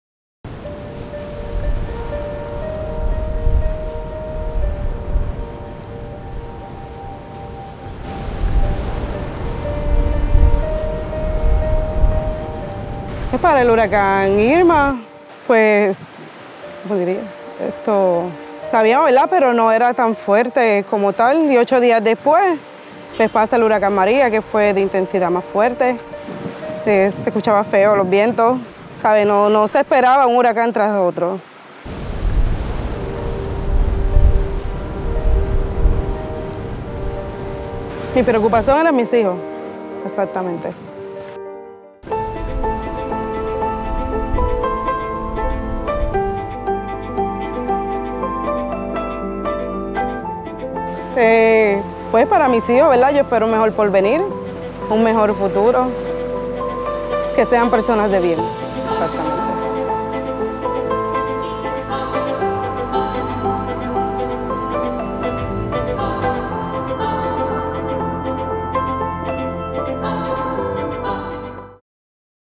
Support Victims of Disaster
Glorimar lost her home and her future in a cyclone. Every day countless people like Glorimar face injuries, sickness, unsafe water, and lose the ability to make a living as a result of disasters.

YOU CAN HELP NOW - ADRA.org/disaster-sm